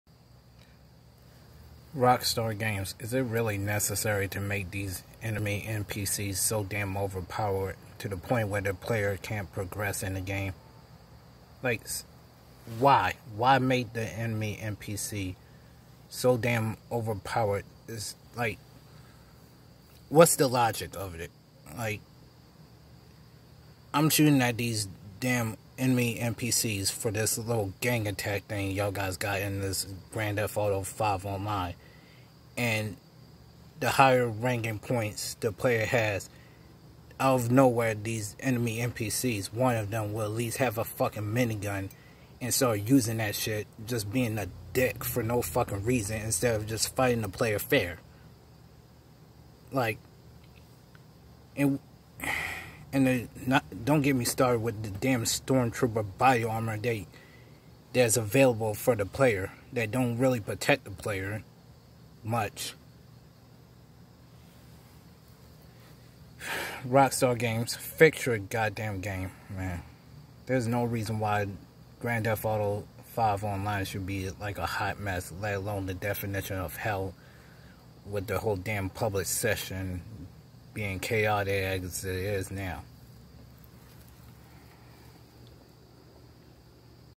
Message to Rockstar Games "Fix GTA 5 online!"
Forget your shark card sales for a moment, it's not a good look on y'all if you make the game unplayable! 😒😠😡🤬